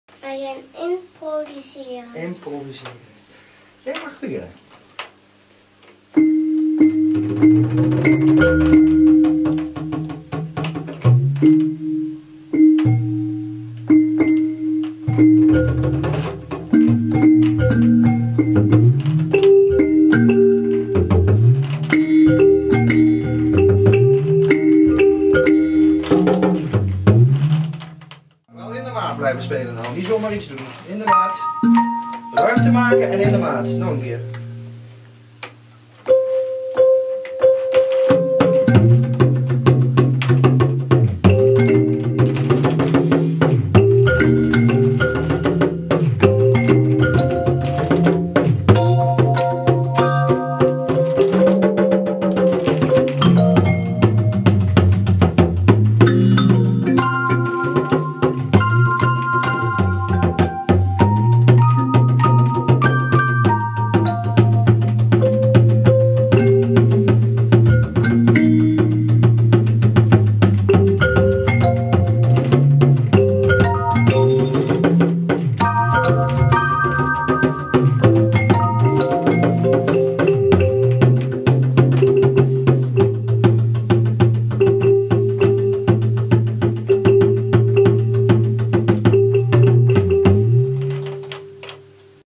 Meet Talkingdrum / Zendrum
The Talkingdrum, allows you to change its pitch, by squeezing it. The Zendrum is a very sensitive midi controller. It allows you to trigger beautiful sounds without any special playing technic. A nice instrument e.g. for children to freely fantasize and improvise with sounds. In this case, once more, my own little girl, trying it out for the first time.